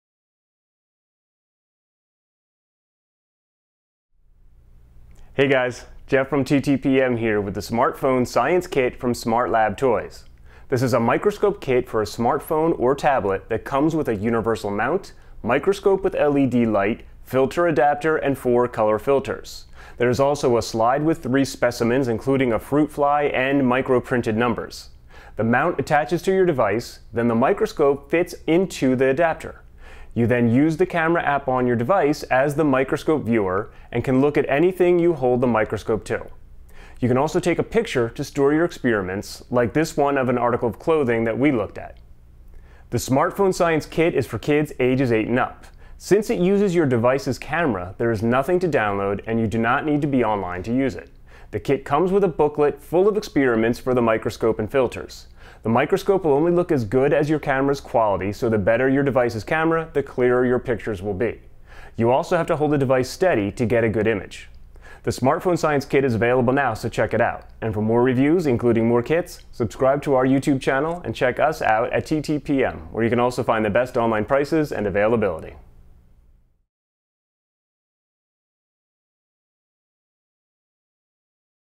SmartPhone Science from SmartLab
The Smartphone Science kit turns a smartphone or tablet into a microscope. For full review and shopping info

Product Info:
The Smartphone Science kit turns a smartphone or tablet into a microscope. The kit includes a universal mount, microscope with LED light, filter adapter and four color filters. There is also a slide with three specimens.

The mount attaches to your device and the microscope fits into the adapter. You then use the camera app on your device as the microscope's viewer. The camera app can also be used to take a picture of the microscopic item so kids can store their experiments.